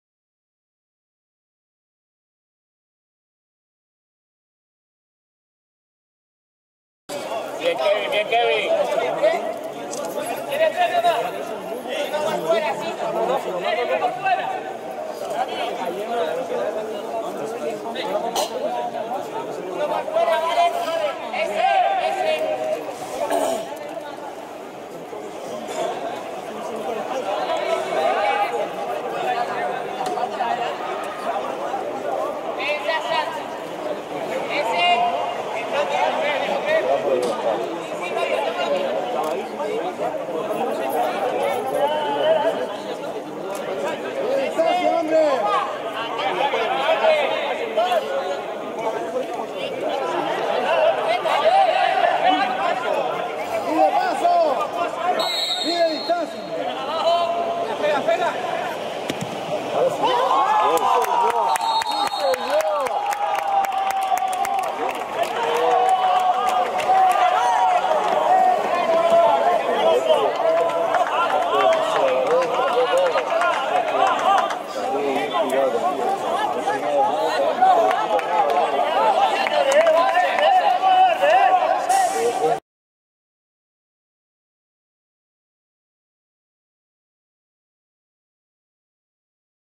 Gol de Simón, jugador del San Pedro Mártir CD 13 11 2015
Gol de Simón,  jugador del San Pedro Mártir CD, minuto 22 (primera parte) 0-1. Partido de fútbol Hespérides CF-San Pedro Mártir CD.
Categoría Regional Preferente Las Palmas. Temporada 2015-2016. Jornada 11. 13/11/2015. Campo de Fútbol de la Junta de Obras del Puerto. Las Palmas de Gran Canaria. Resultado final: 2-1.